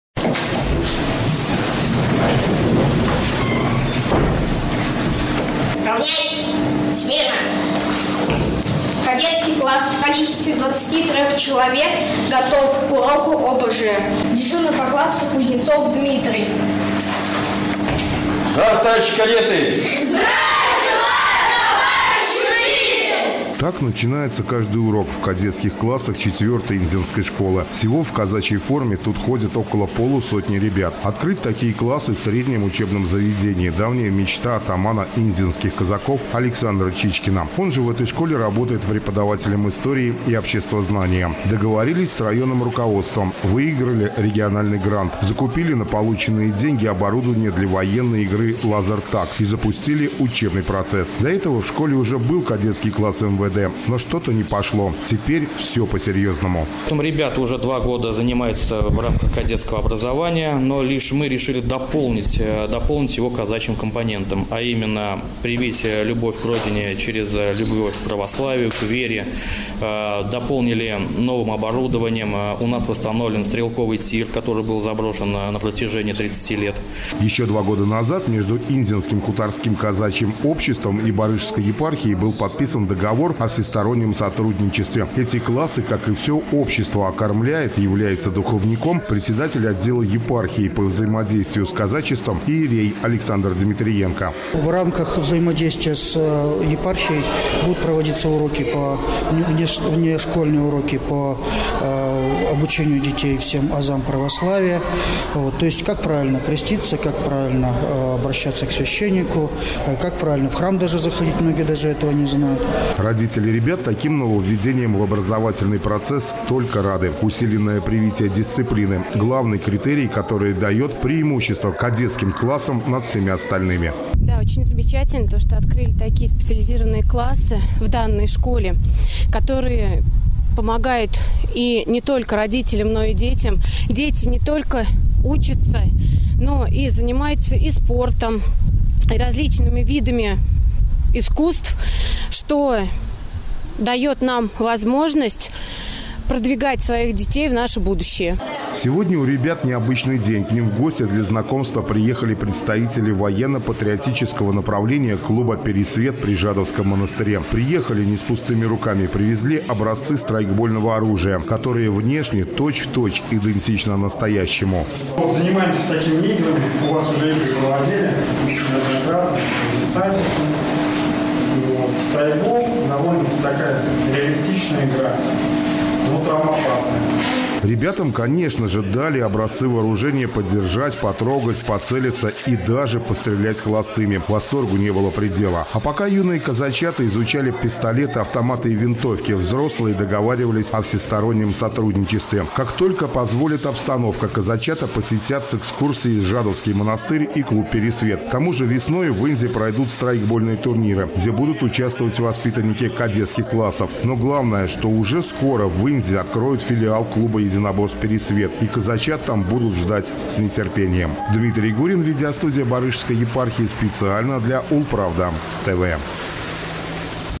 Дисциплина, закон и оружие. В инзенской школе открыли необычные классы
В 4-й школе Инзы начали работать два казачьих кадетских класса 5-й А и 7-й А. Наряду с общеобразовательными предметами ребята и девушки на внеклассных занятиях будут изучать «Военное дело» и «Закон Божий».